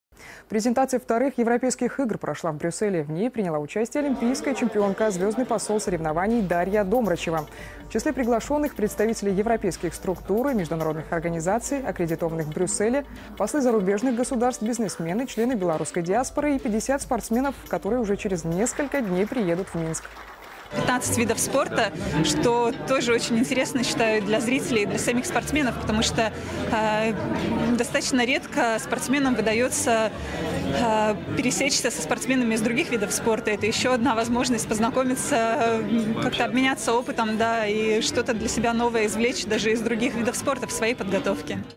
Дарья Домрачева приняла участие в презентации II Европейских игр в Брюсселе
Дарья Домрачева: "15 видов спорта, что тоже, я считаю, очень интересно для зрителей, для самих спортсменов, потому что достаточно редко спортсменам выдается пересечься со спортсменами из других видов спорта. Это еще одна возможность познакомиться, как-то обменяться опытом, что-то для себя новое извлечь из других видов спорта даже в своей подготовке".